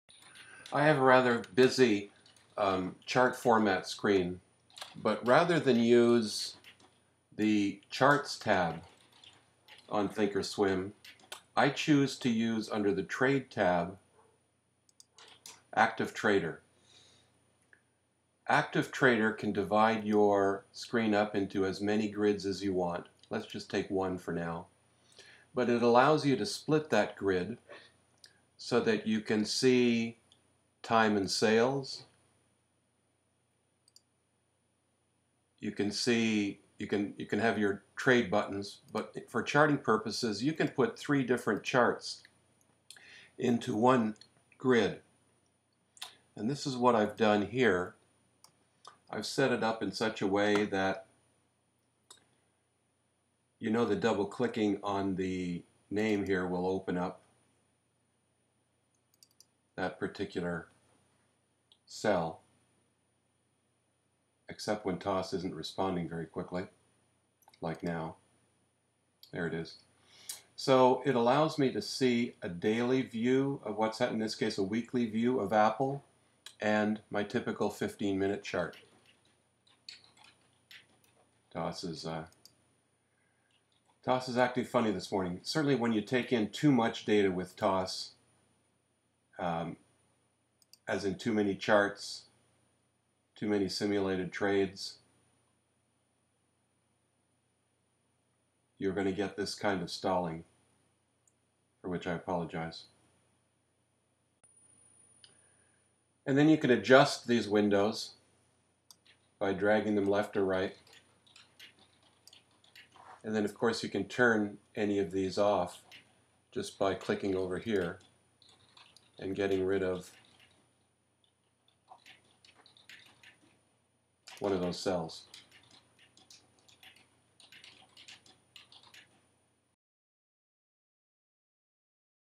Using Active Trader for your charts
Quick Tip: How to use Active Trader for your TOS charts instead of the Chart tab